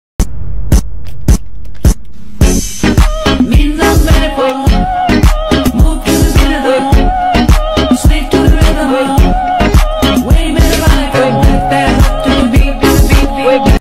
2023 Skincare Shelf Organization
There's only 2 months left of 2023, lets take a look at our skincare shelf!